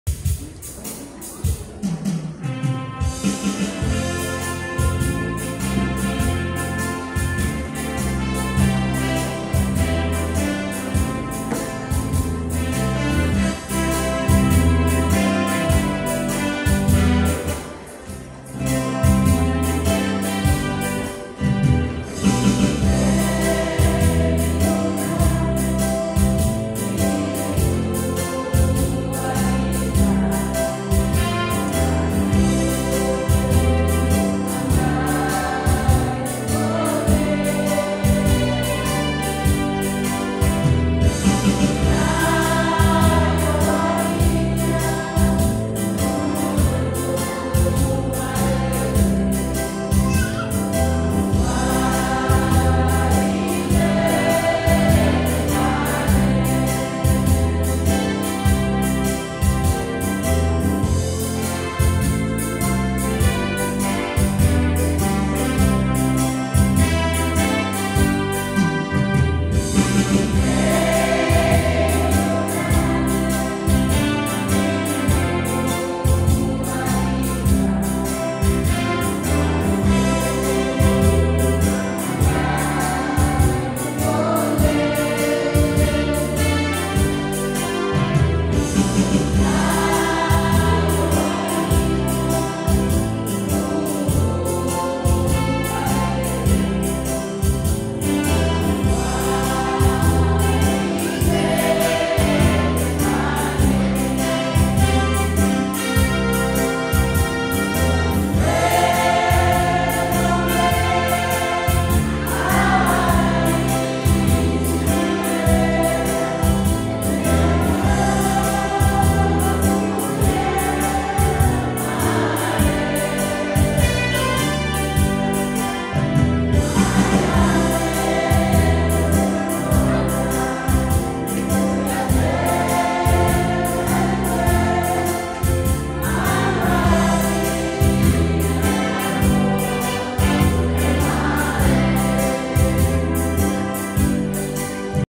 Vokal Grup TABI
Ibadah HUT GKI di Tanah Papua yang ke-68 tahun.